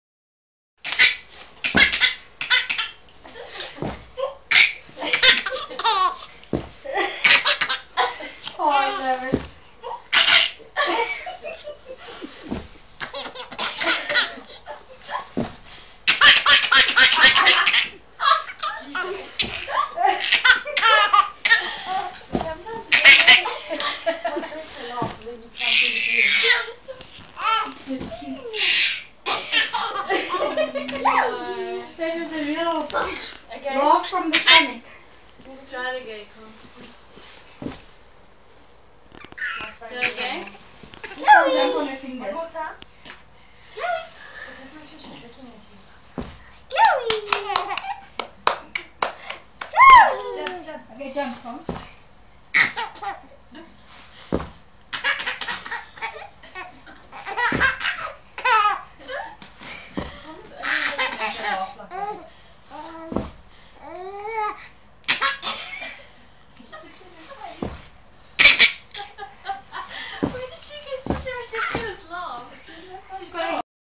Murray Kuun chloe video.mp4
Murray Kuun. Our Chloe laughing at her older sister, our Carmi